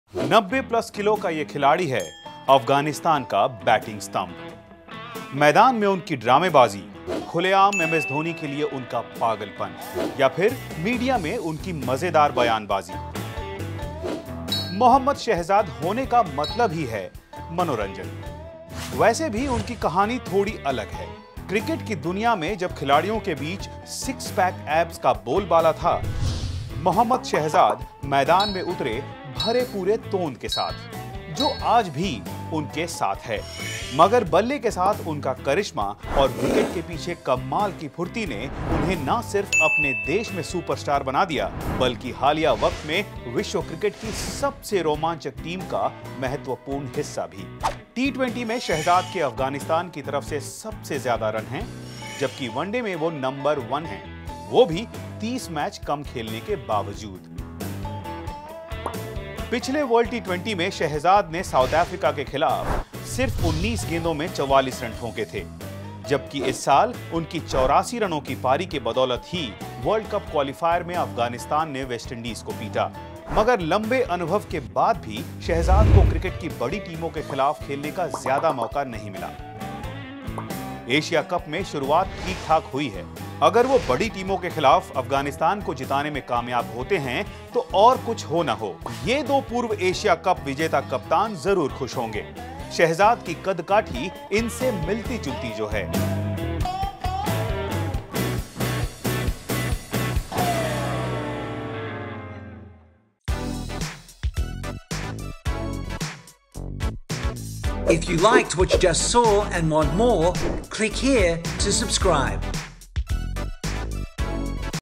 ESPN Shorts: Mohammad Shahzad (Hindi)
Can this “heavy” hitter bring home the Asia Cup for Afghanistan?